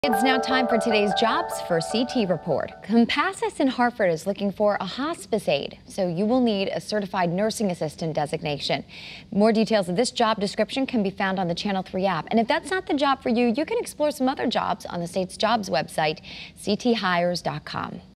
Jobs for CT: Hospice aide
Compassus in Hartford is looking for a hospice aide.